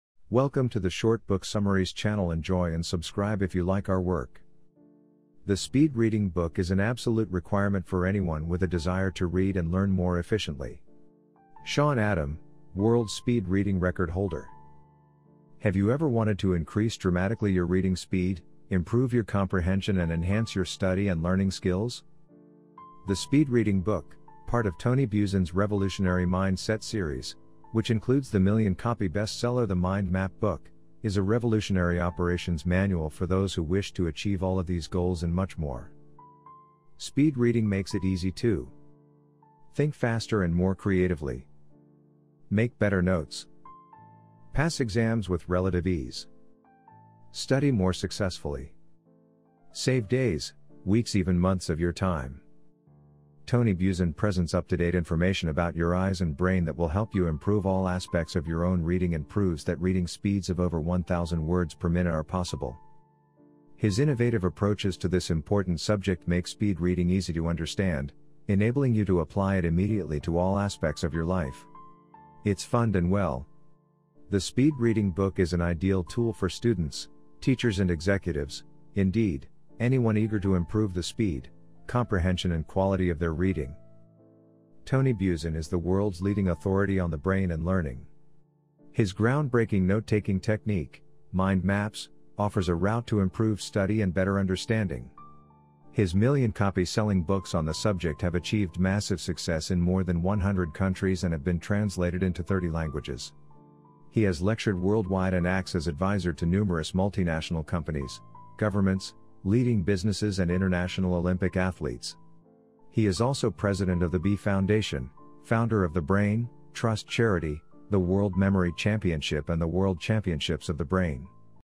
Short Book Summary of The Speed Reading Book Mind Set by Tony Buzan
'The Speed Reading Book' is an absolute requirement for anyone with a desire to read and learn more efficiently.' Sean Adam, World Speed Reading Record Holder.

Have you ever wanted to increase dramatically your reading speed, improve your comprehension and enhance your study and learning skills?

The Speed Reading Book, part of Tony Buzan's revolutionary 'Mind Set' series (which includes the million-copy bestseller The Mind Map" Book), is a revolutionary operations manual for those who wish to achieve all of these goals and much more!

Speed Reading makes it easy to:
- think faster and more creatively
- make better notes
- pass exams with relative ease
- study more successfully
- save days, weeks even months of your time.

Tony Buzan presents up-to-date information about your eyes and brain that will help you improve all aspects of your own reading and proves that reading speeds of over 1,000 words per minute are possible.

His innovative approaches to this important subject make speed reading easy to understand, enabling you to apply it immediately to all aspects of your life. It's fund and well! The Speed Reading Book is an ideal tool for students, teachers and executives - indeed, anyone eager to improve the speed, comprehension and quality of their reading.

Tony Buzan is the world's leading authority on the brain and learning. His ground-breaking note-taking technique, Mind Maps, offers a route to improved study and better understanding. His million-copy selling books on the subject have achieved massive success in more than 100 countries and have been translated into 30 languages. He has lectured worldwide and acts as advisor to numerous multi-national companies, governments, leading businesses and international Olympic athletes. He is also President of the B Foundation, founder of the Brain, Trust Charity, the World Memory Championship and the World Championships of the Brain.